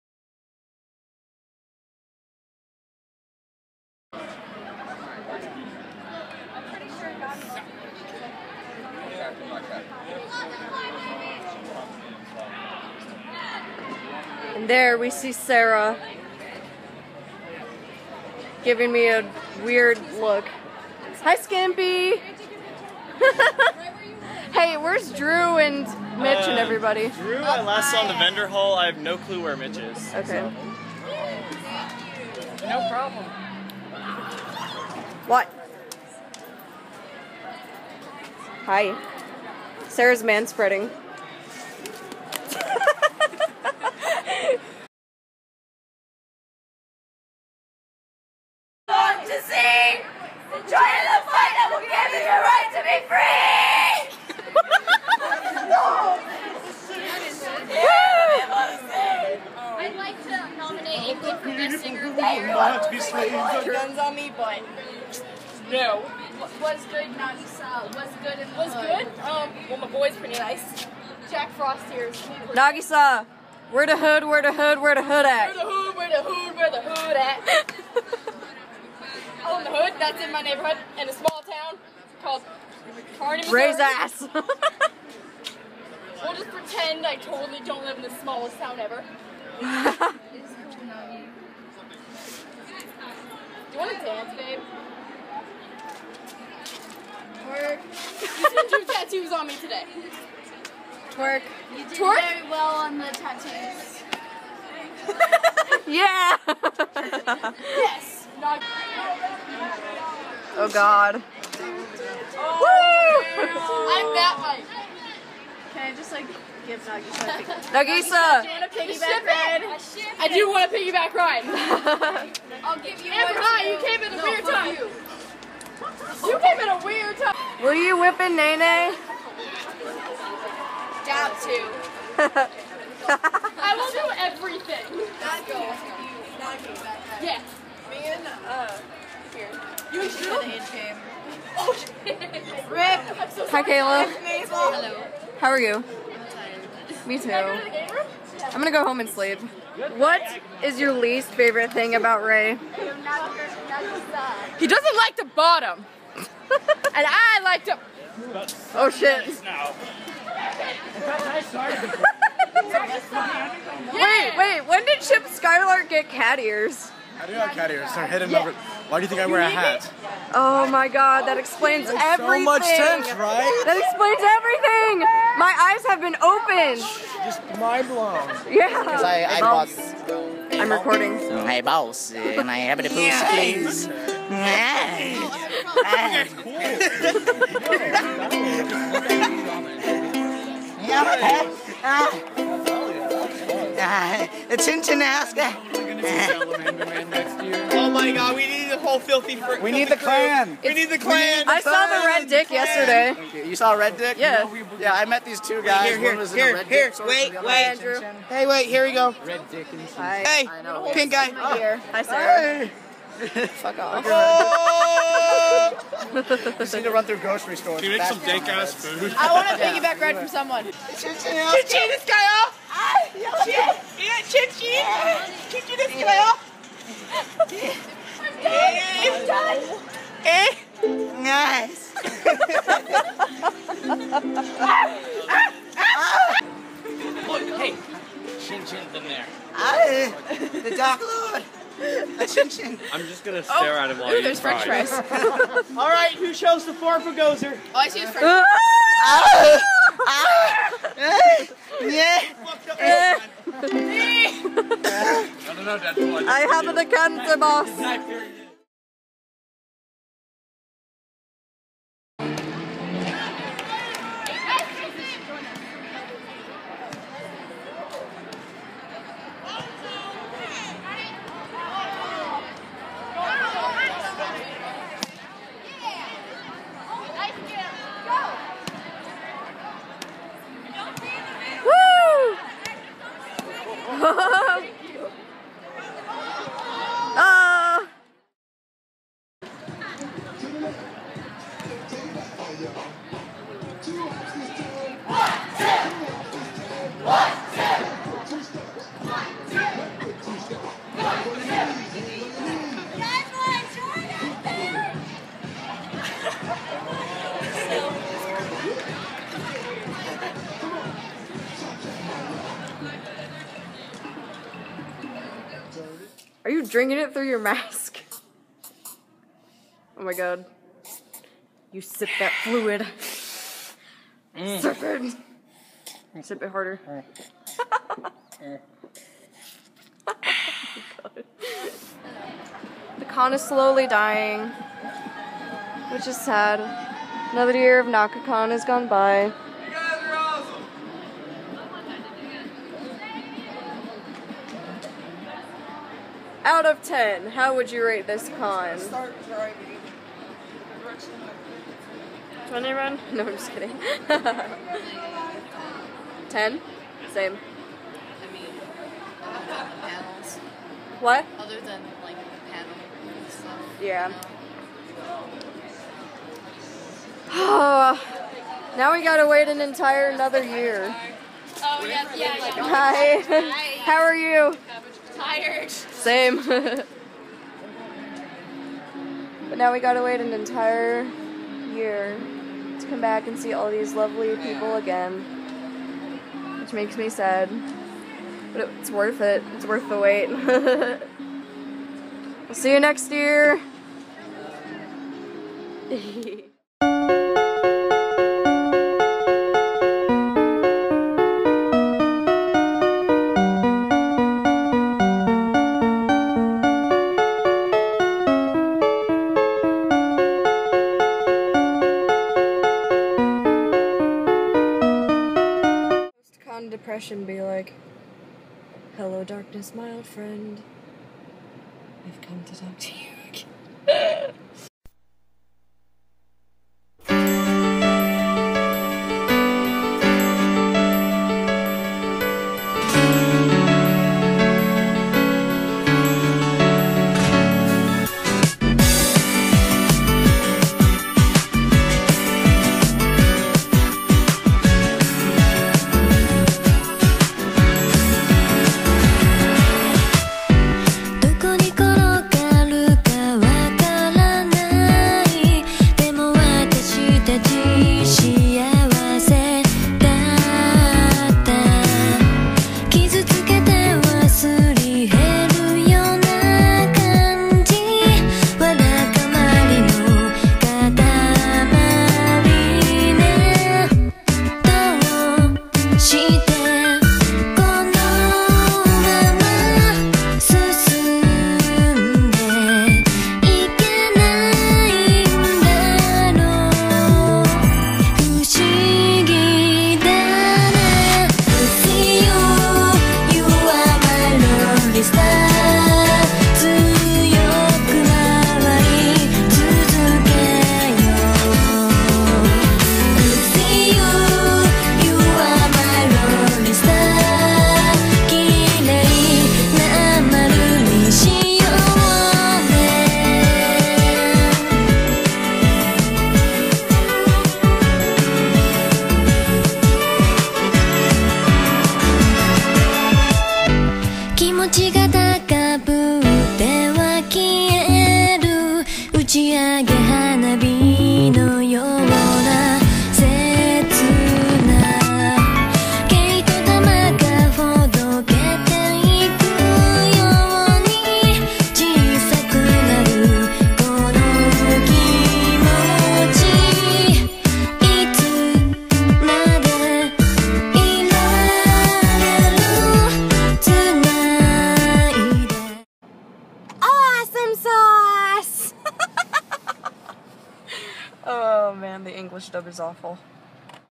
Naka-Kon 2016 Vlog #6 | Day Three: The Third One
It may not have been as interesting as the past 2 days, but it was still a day to remember. Let the post-con depression commence.

The Defense Squad is back for our 3rd Naka Kon and 2nd year of vlog adventures!
Special shoutouts to all the people I forgot to give shoutouts to xD as well as the lovely people and cosplayers shown in this vlog.

GBDS is Kat, Kayla, and their friends.